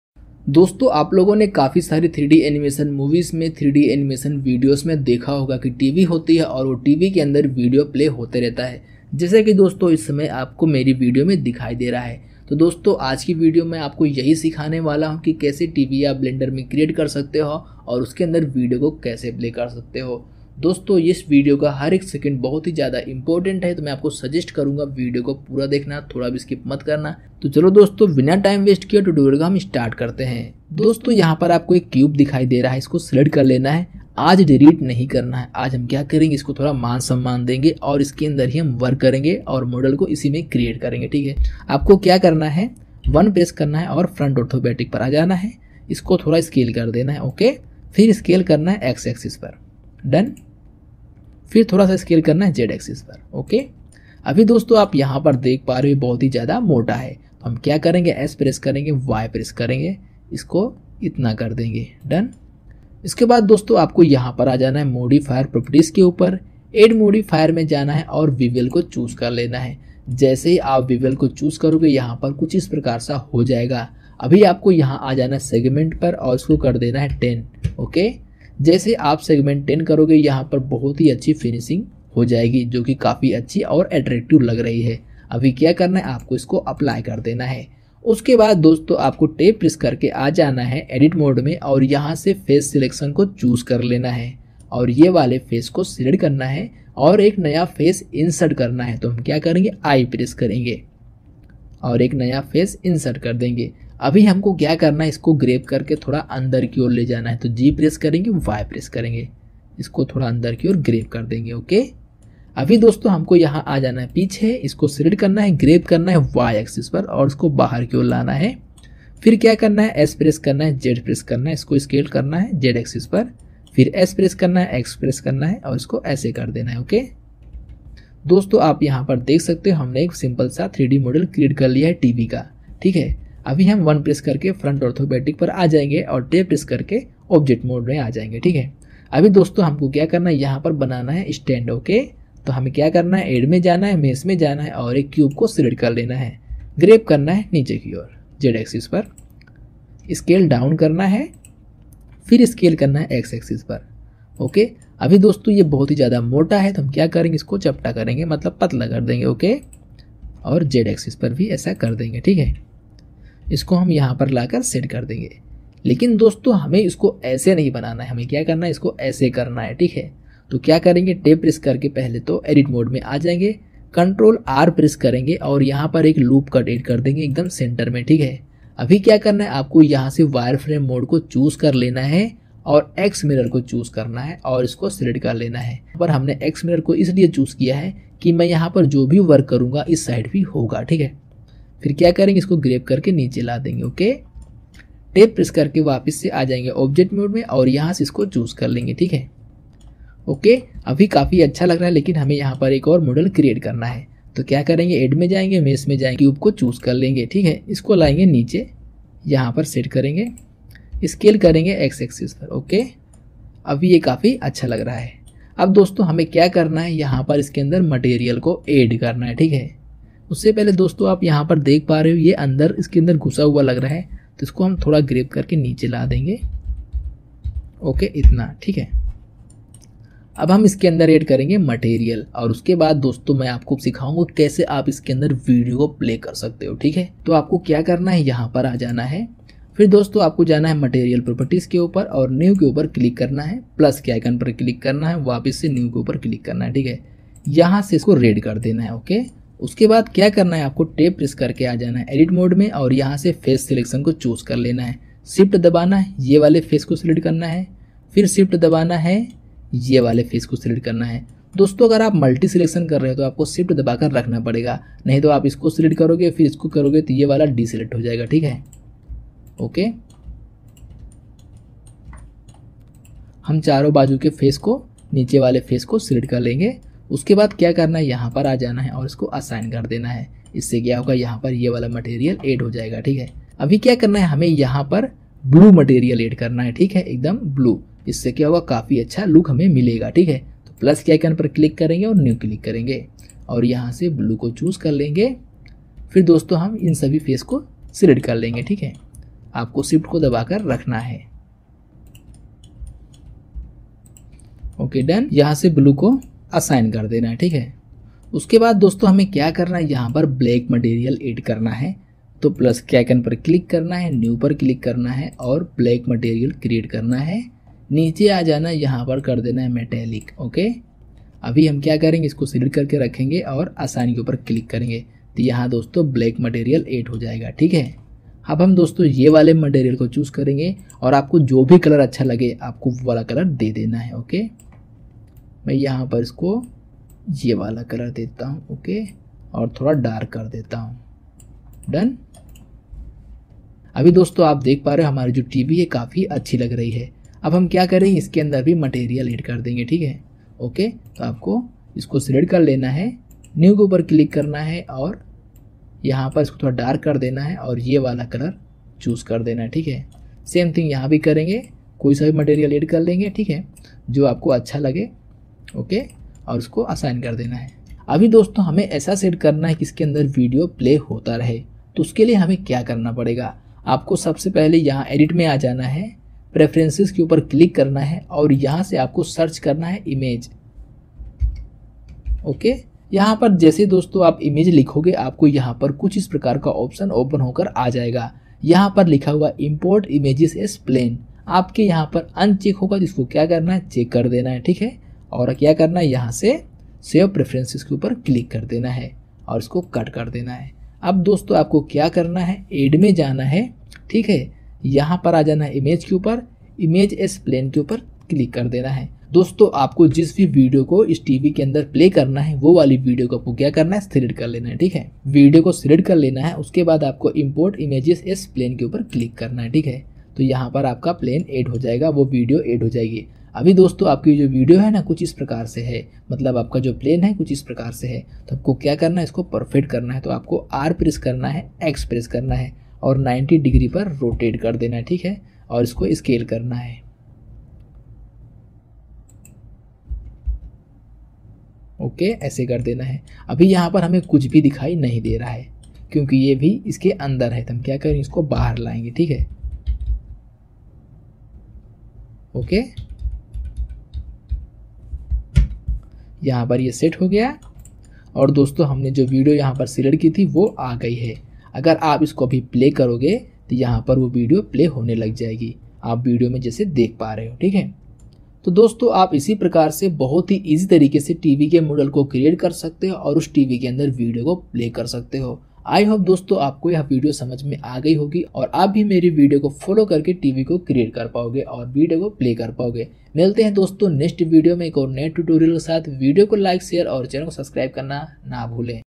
how to make video playing 3d tv in blender-blender hindi animation tutorial
hello friend in this video we will make 3d video playing tv in blender